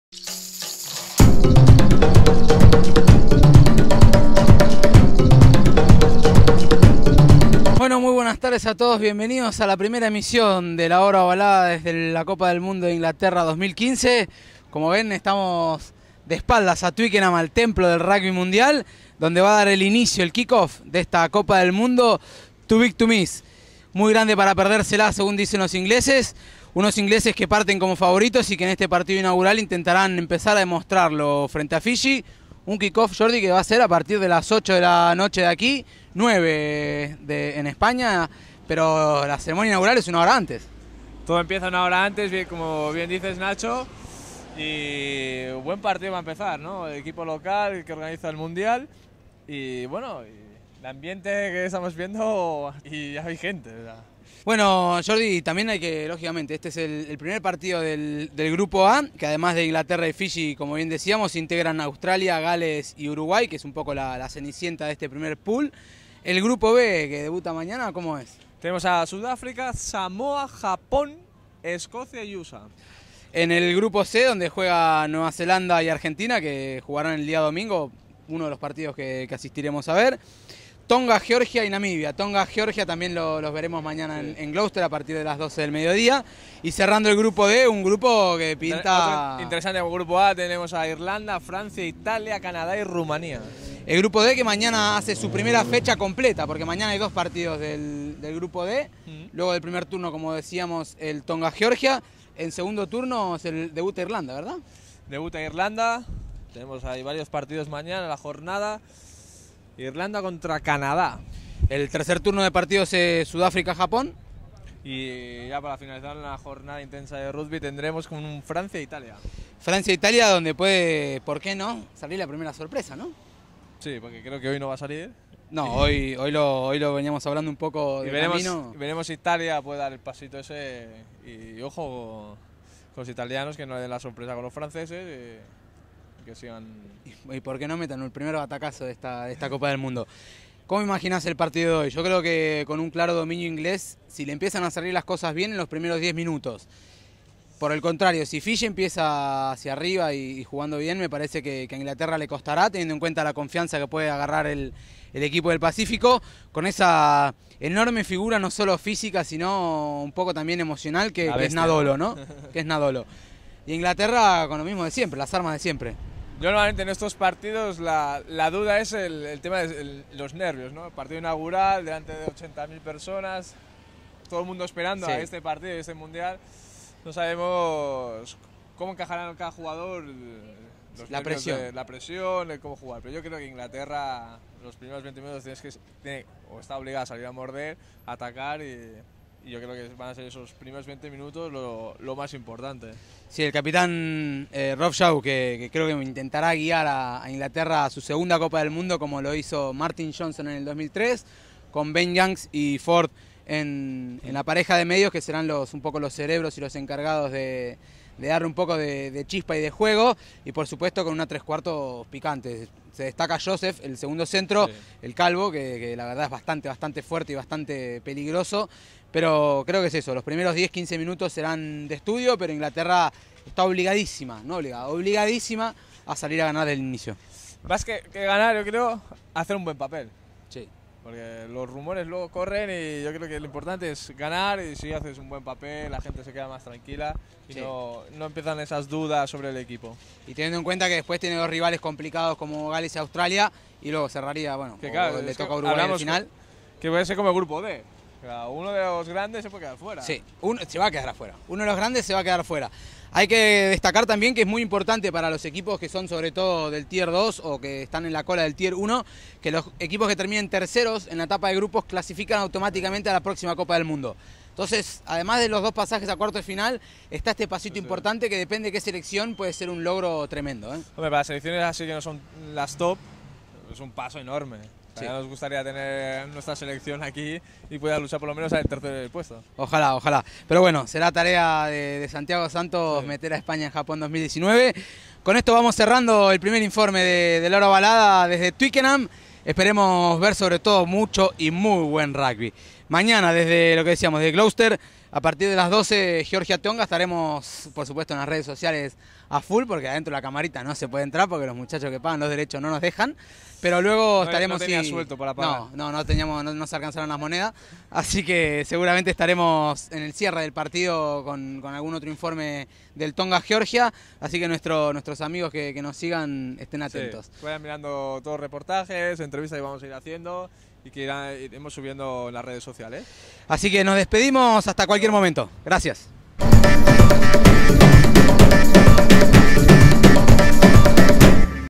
La Hora Ovalada RWC2015 Day 01
Primer informe de la Rugby World Cup de La Hora Ovalada, desde Twickenham, con el análisis previo del partido inaugural entre Inglaterra y Fiji, con Jordi Ribot y Nacho Arla como enviados especiales.

Lo mejor del Rugby al día y en imágenes de alta definición